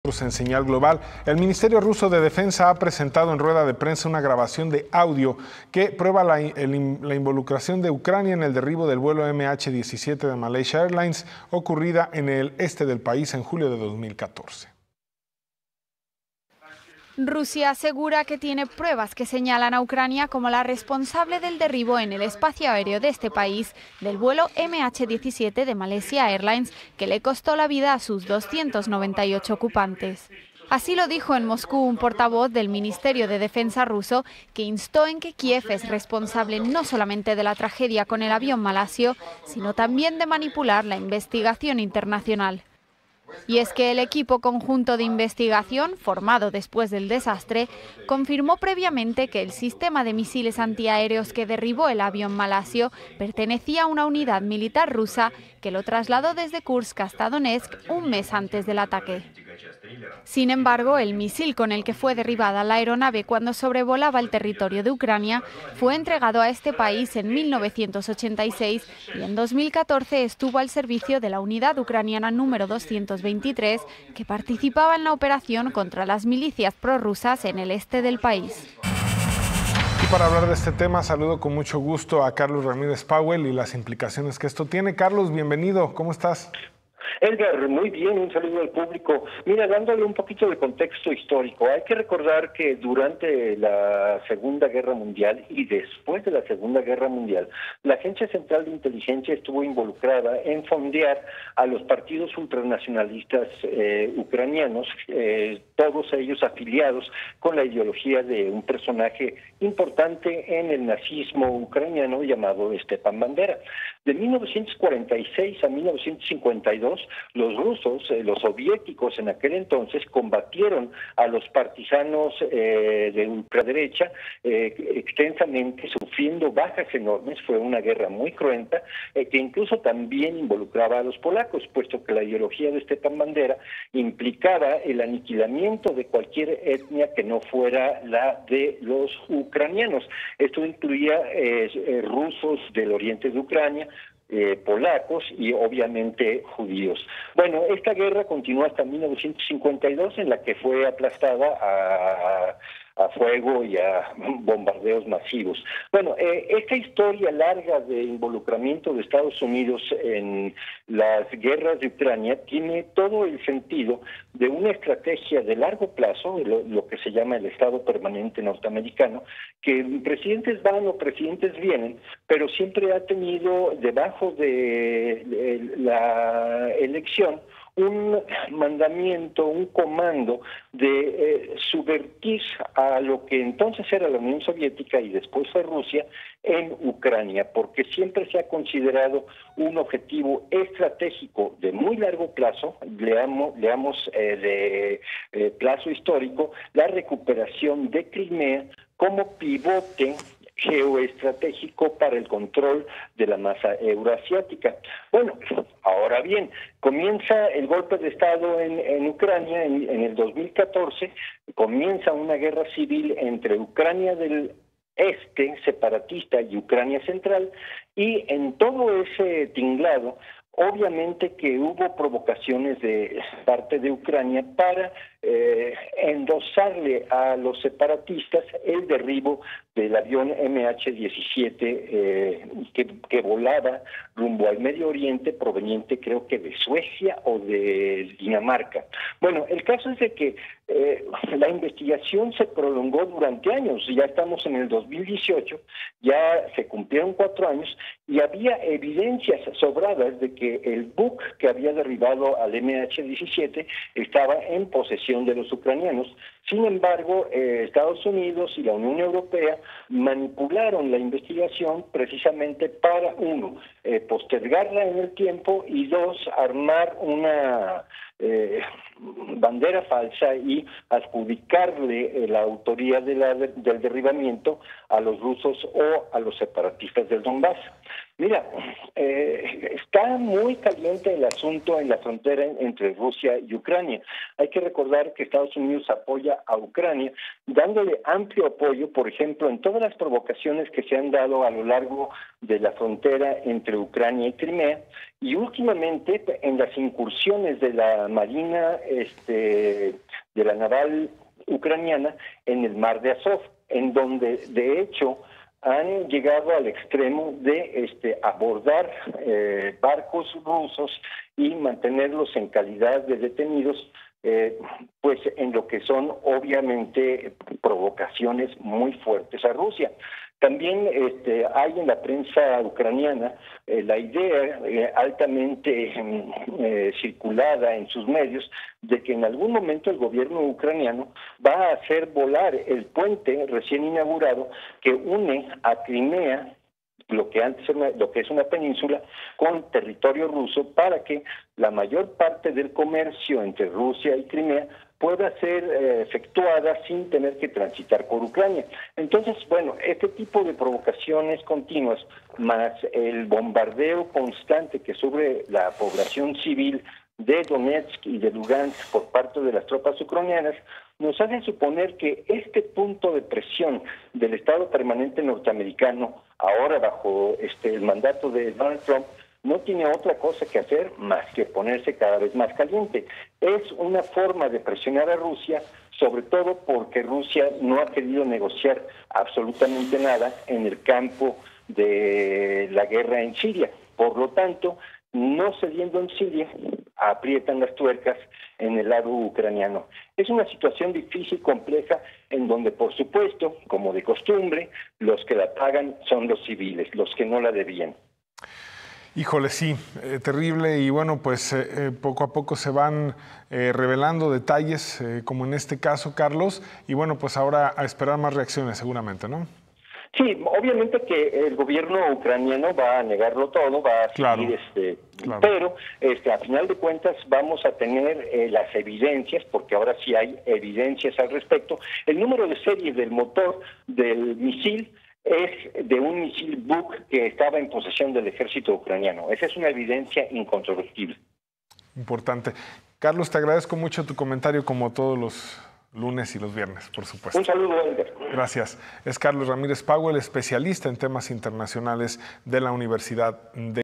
Carlos Ramírez Powell - EEUU involucrado en los conflictos de Ucrania
¿Estados Unidos tiene que ver con los últimos conflictos ucranianos? ¿O será que el interés es sobre  Donetsk y Luhansk como “regiones rebeldes”?

El tema que nos trae hoy @Pleiotropik en SeñalGlobal